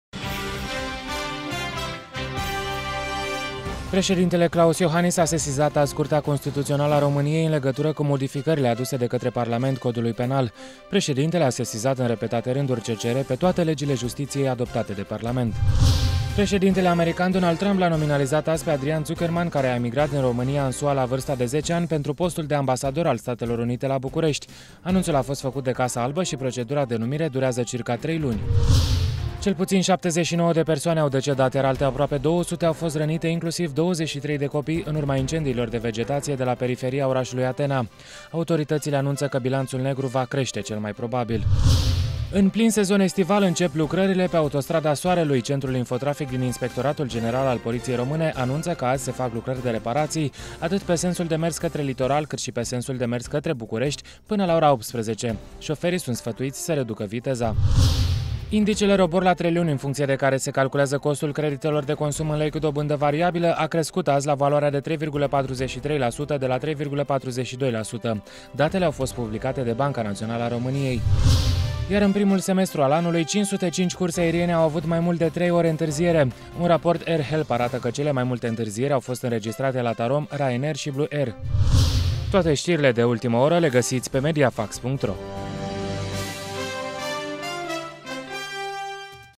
Stiri Mediafax 250718 set 2
Știrile prânzului: : Klaus Iohannis sesizează CCR în legătură cu modificările Codului Penal .Adrian Zuckerman a fost nominalizat pe postul de ambasador al Statelor Unite în România.: Bilanţul incendiilor de vegetaţie din Grecia a ajuns la cel puţin 79 de morţi